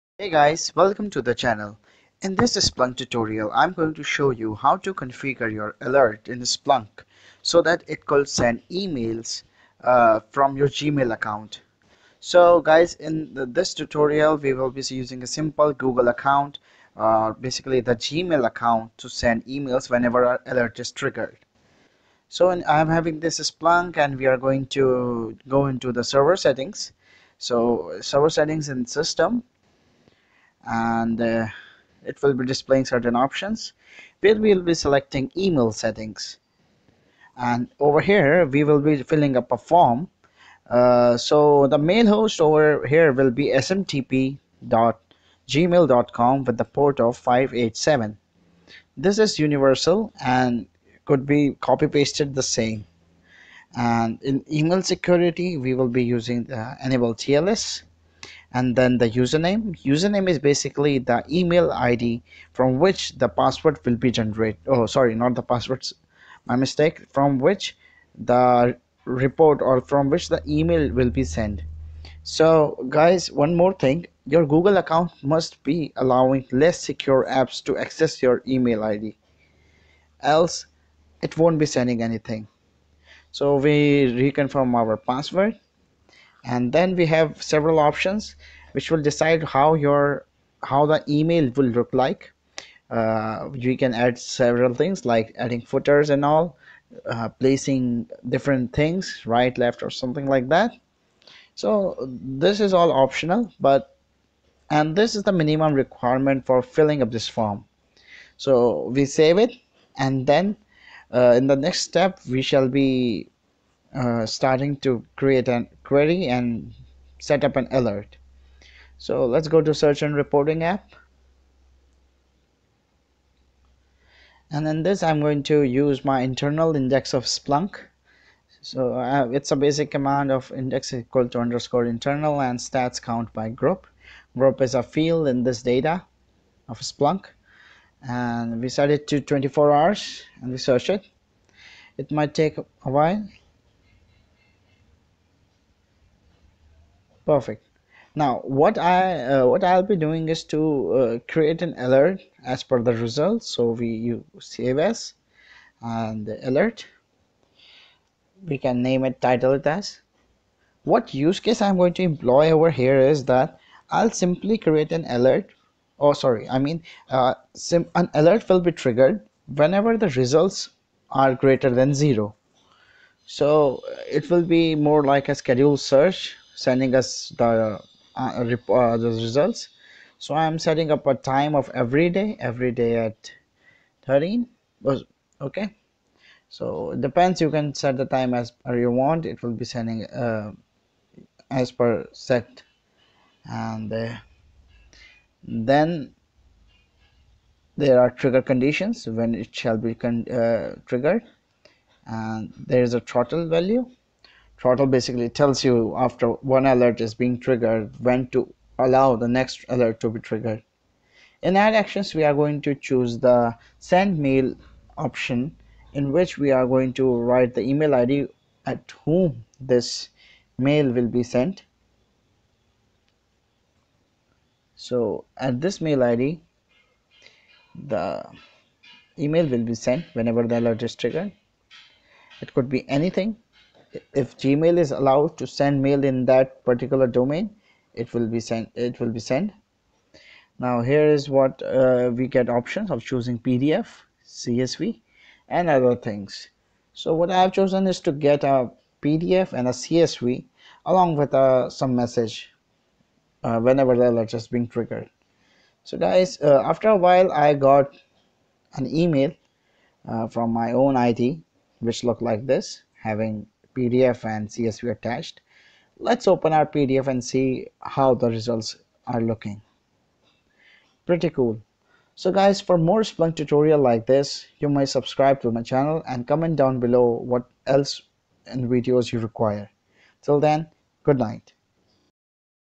How to send mails from splunk by gmail account | Configuring mails in alert
In this Splunk tutorial we have shown how to configure an alert which can send mails through your Gmail account.
In this video, we can also learn , that how to configure an Alert.

How to allow less secure apps to access google account: -